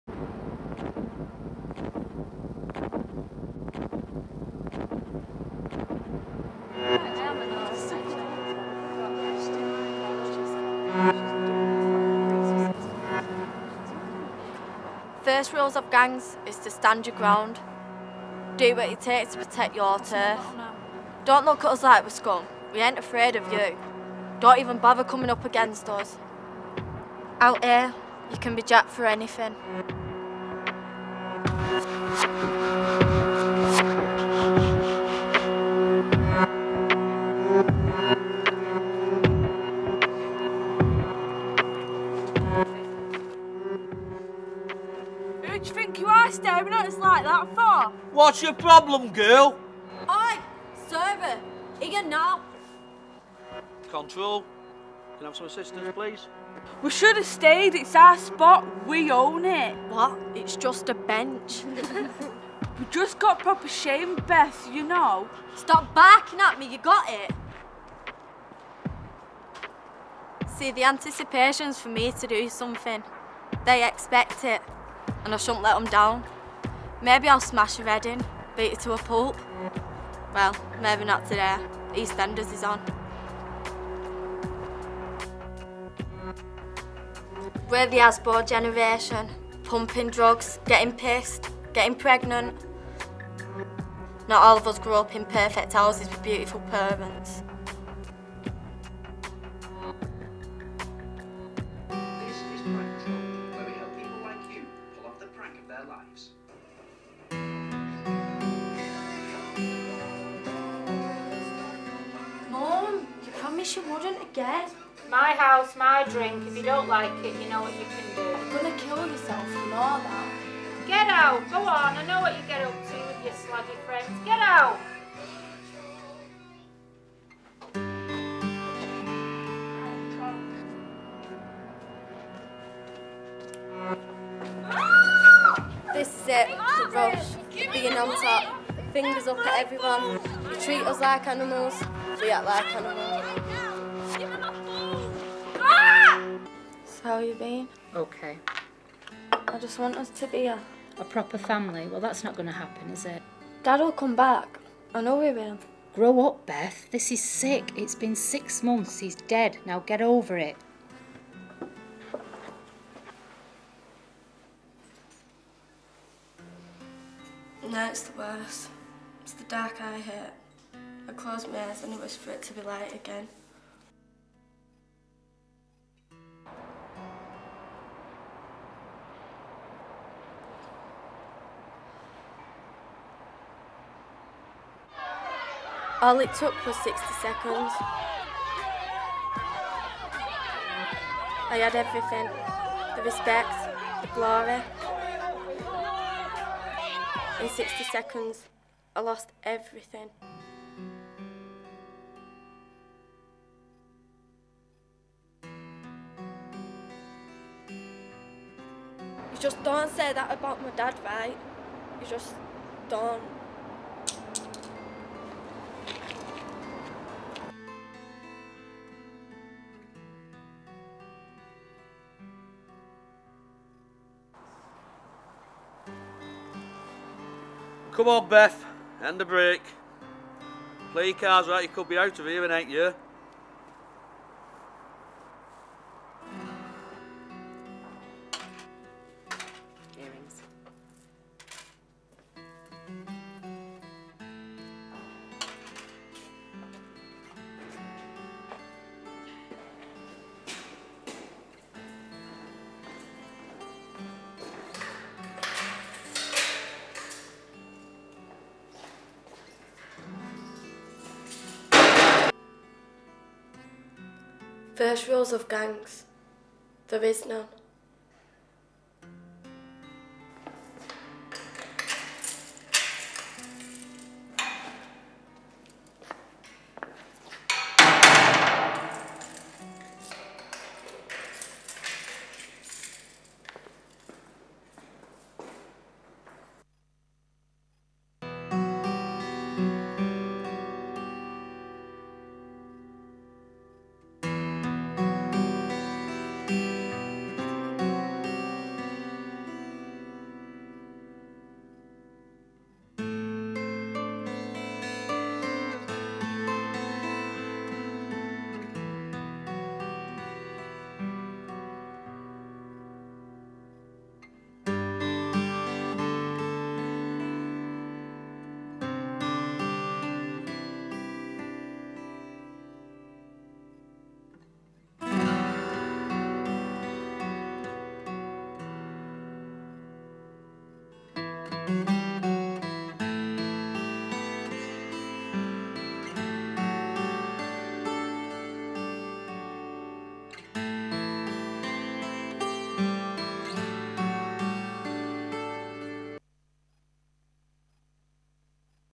Clapperboard 2009 - Beth's Story
Film devised by the First Diploma Media Students from the Oldham College. Produced in association with Clapperboard